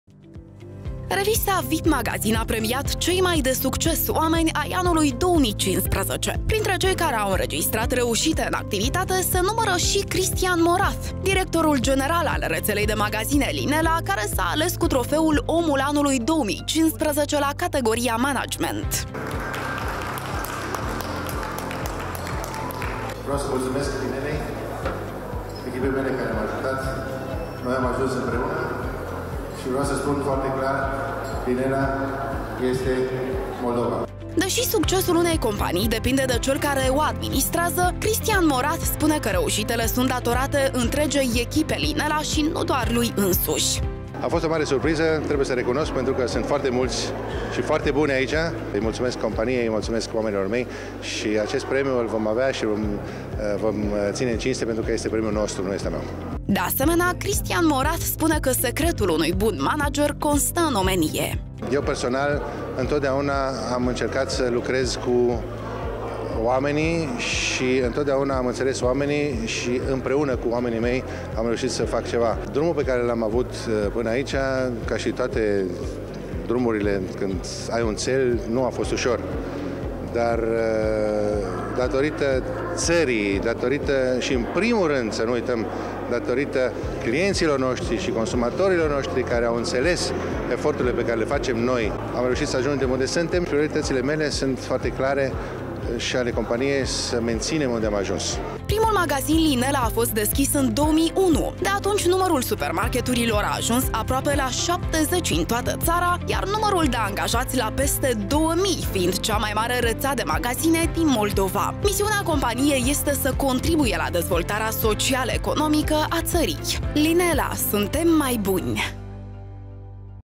Directorul general Linella - Omul Anului 2015 la categoria management
Revista VIP MAGAZIN a premiat cei mai de succes oameni al anului 2015.Directorul general Linella, domnul Cristian Morath, a obținut premiul OMUL ANULUI  la categoria management!